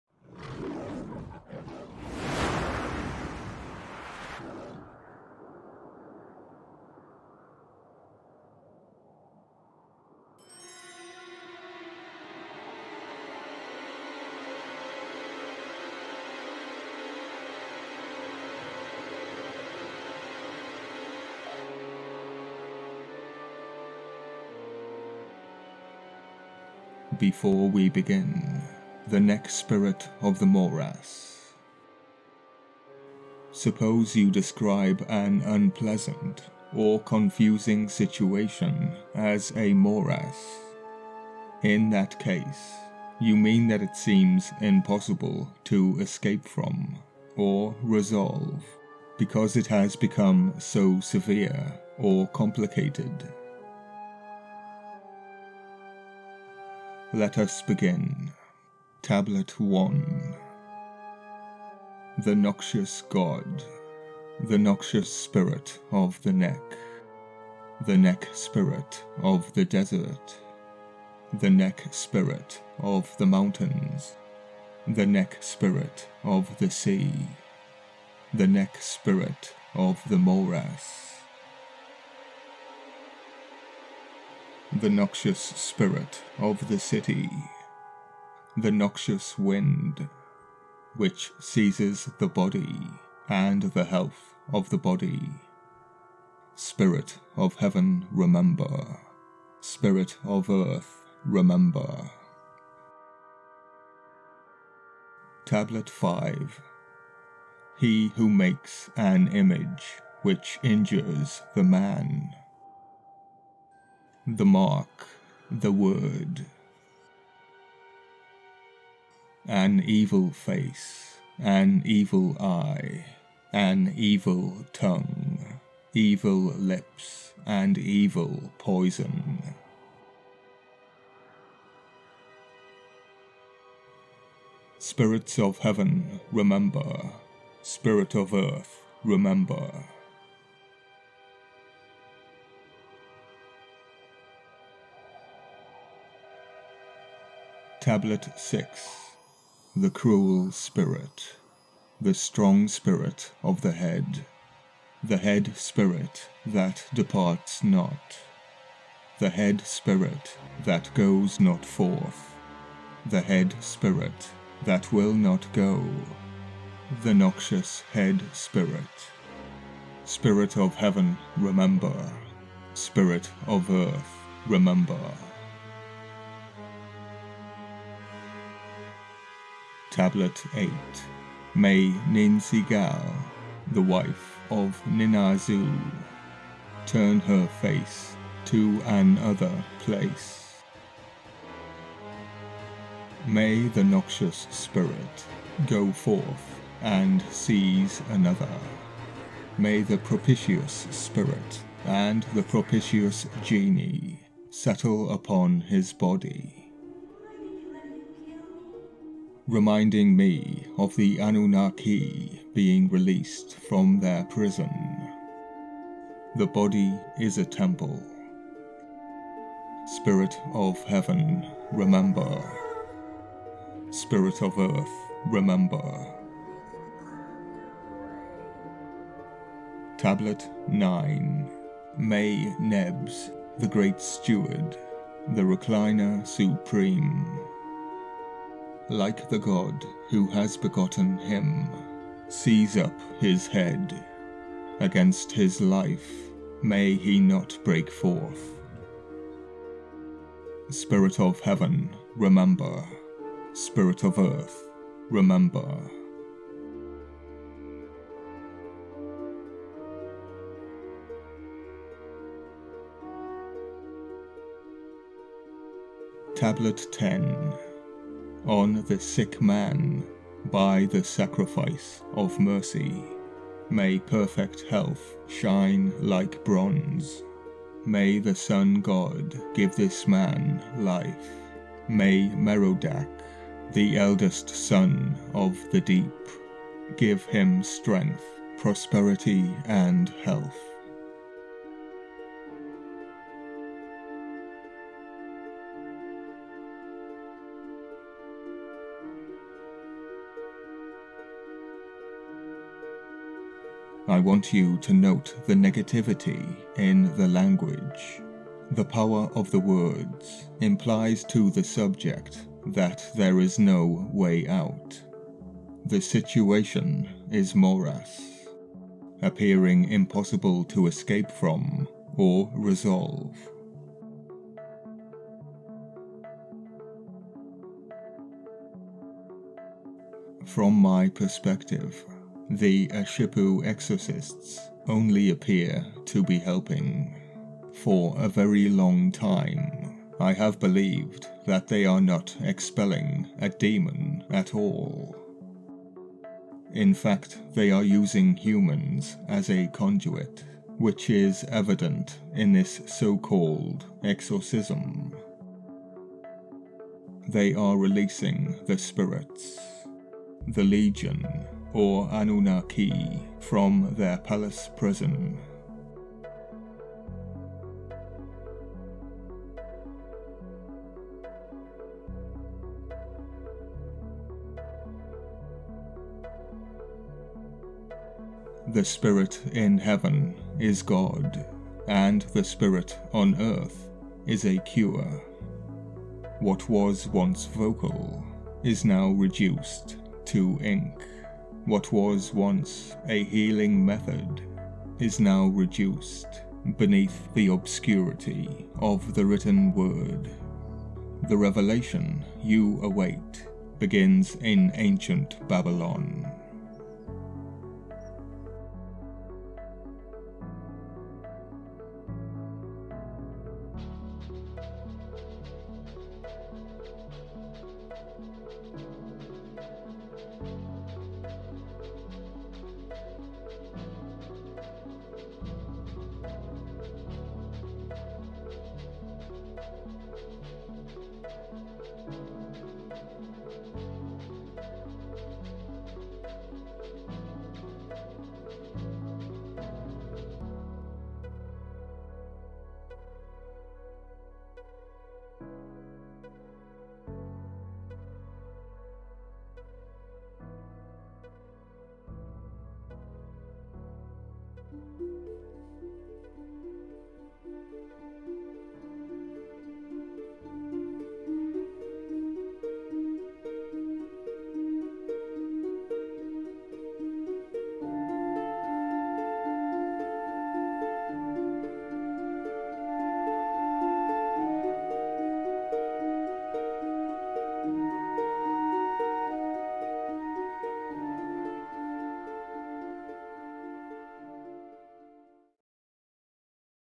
A Babylonian Exorcism of the Spirits of Disease - Asipu Rituals
The Exorcism of the Spirits of disease. The Babylonian Tablets used by the Asipu (exorcists) as part of a magical healing ritual performed upon the people with an affliction, disease, or worse. The Ancient Mystery of the day.

The Prayer for Health is one of many elements of the ritual;
the Tablet also refers to a noxious spirit of the neck, two propitious spirits, the Spirit in Heaven and the Spirit on Earth, Marduk (Merodach) and the unnamed Sun god.

The exorcisms in ancient Babylon were far from uncommon, but what is the truth behind this dark and spooky ritual? Join me @ while I lift the veil upon the world of magic and sorcery.

Channel: Ancient Mystery by A. Christie